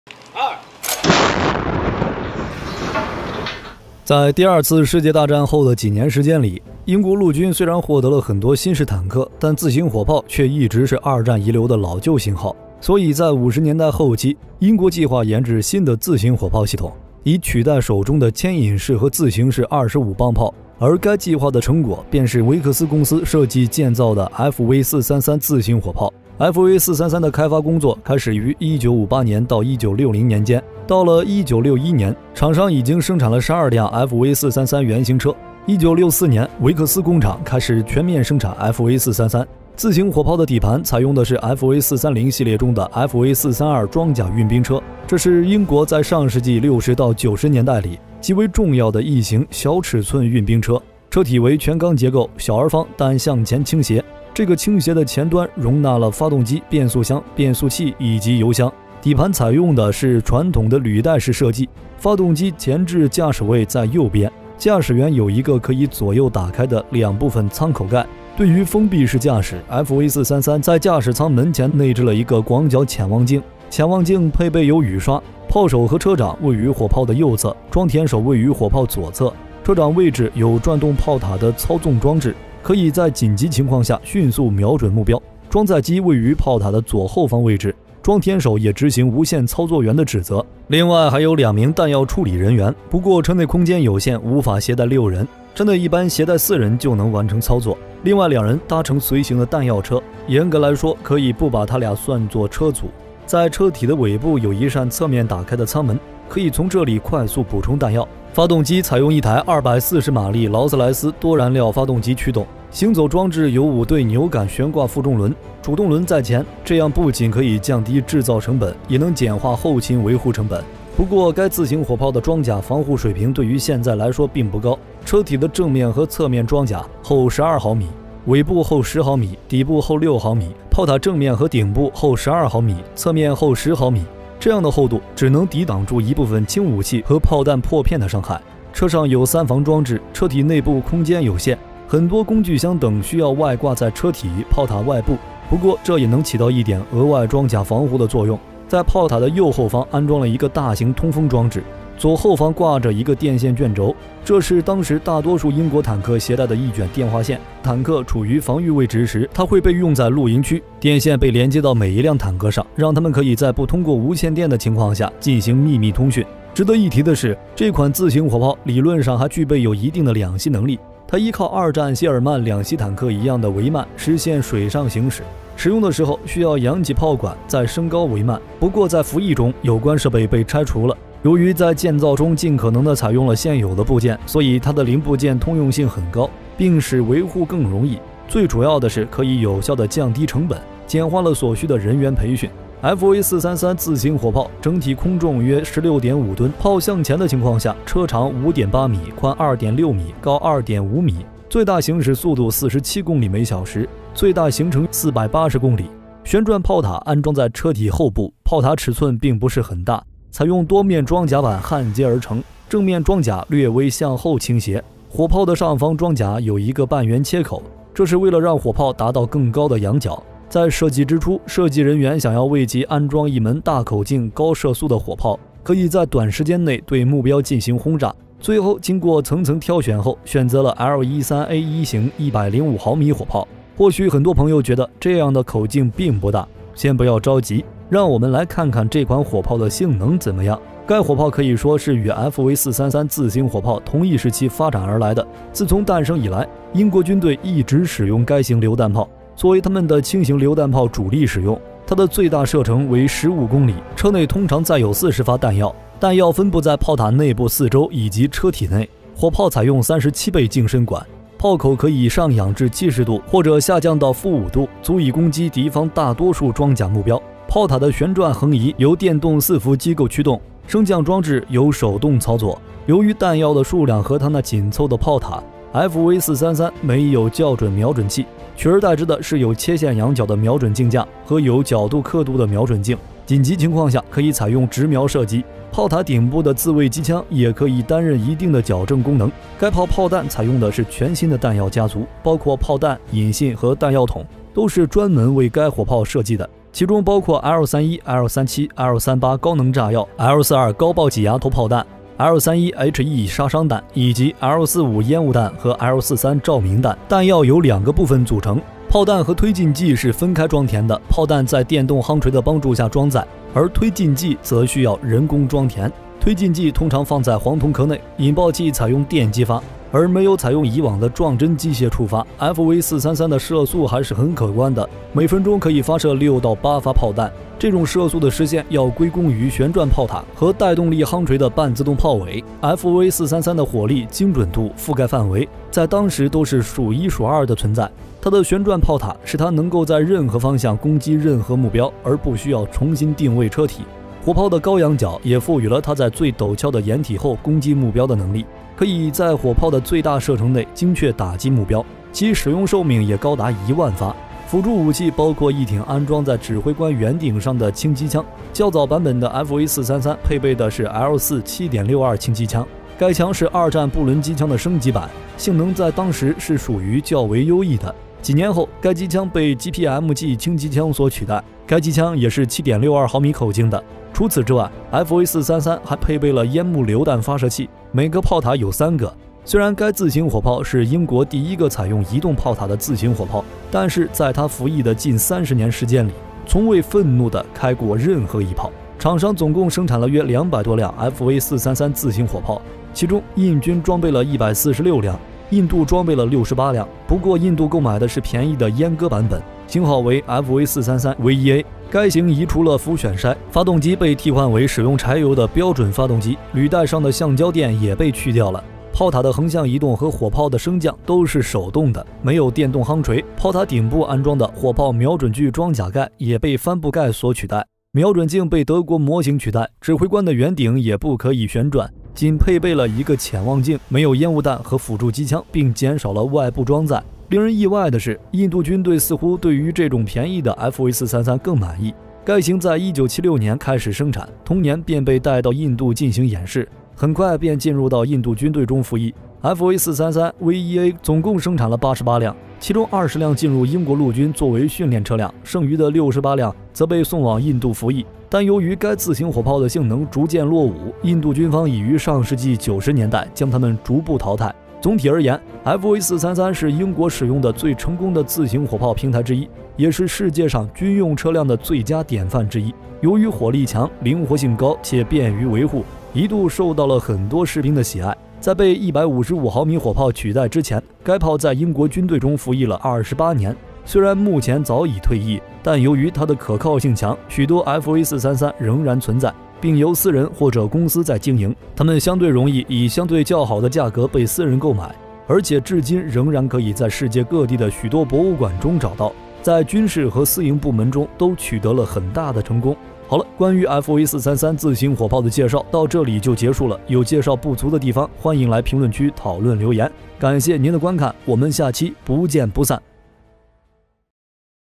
【FV433自行火砲】英國最小自行榴彈炮，短小精悍性能超越大多數同時期火砲，印度卻只對閹割版情有獨鐘，如今大多流入民間
FV433自行火砲空重約16.5噸，車長5.8米（炮向前 ），寬2.6米，高2.5米，最大行駛速度47千米每小時，最大行程480千米。
自行火砲內部彈藥架可以攜帶42發分裝彈藥，它們分佈在砲塔內部四周以及車體內，火砲採用37倍徑身管，砲塔旋轉為電動，火砲俯仰則為手動。彈藥的主要型號有HE、HESH、煙霧彈、照明彈。發射藥被放在黃銅彈殼中使用，射手可以根據射擊需要選擇裝填多少包發射藥，以及選擇常規彈殼還是加壓彈殼，在使用強裝藥時最大射程可以達到17.3千米。 FV433緊急情況下可以直瞄射擊，不過因為車輛太緊湊沒有相關的光學設備，因此砲塔頂部的自衛機槍擔任起了矯正的功能，早期安裝的是一挺布倫機槍升級版型號，後來被其它的7.62毫米機槍替換。 FV433的裝甲防護偏弱，車體的正面和側面裝甲厚12毫米，尾部厚10毫米，底部厚6毫米，砲塔正面和頂部厚12毫米，側面厚10毫米，只能抵擋輕武器和砲彈破片的傷害，車上有三防裝置。車輛內部空間有限，很多工具箱等外掛在車體、砲塔外部，起到一點額外的保護。
The FV433 self-propelled artillery has an empty weight of about 16.5 tons, a vehicle length of 5.8 meters (the gun is forward), a width of 2.6 meters, a height of 2.5 meters, a maximum speed of 47 kilometers per hour, and a maximum travel of 480 kilometers.
The internal ammunition rack of the self-propelled artillery can carry 42 rounds of sub-loaded ammunition, which are distributed around the interior of the turret and inside the car body. The main types of ammunition are HE, HESH, smoke bombs, and flares. The propellant is placed in a brass shell, and the shooter can choose how many packets of propellant to load according to the shooting needs, as well as choose a conventional shell or a pressurized shell, and the maximum range can reach 17.3 kilometers when using a strong charge. The FV433 can shoot directly in an emergency, but because the vehicle is too compact and has no relevant optical equipment, the self-defense machine gun on the top of the turret acts as a correction function. An upgraded version of the Bren machine gun was installed in the early stage, which was later used by other 7.62mm machine gun replacement. The armor protection of FV433 is weak. The front and side armor of the hull is 12 mm thick, the tail is 10 mm thick, the bottom is 6 mm thick, the front and top of the turret is 12 mm thick, and the sides are 10 mm thick, which can only withstand the damage of small weapons and shell fragments. , There are three defense devices on the car. The interior space of the vehicle is limited, and many toolboxes are attached to the outside of the vehicle body and turret to provide a little extra protection.